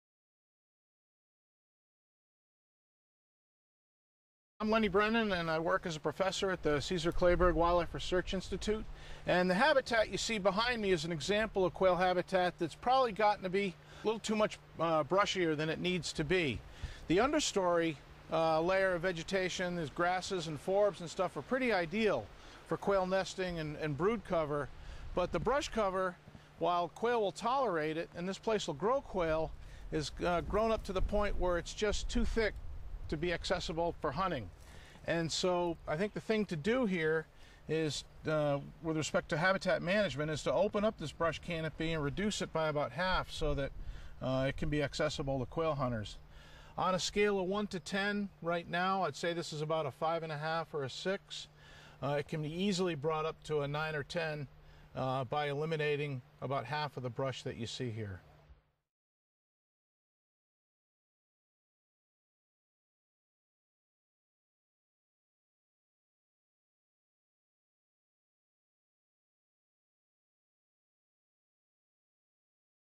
Quail Posts - South Texas Quail Habitat Rating-Brush - Texas Wildlife Association
Dr. Lenny Brennen shows how to rate an area on your land for good quail habitat.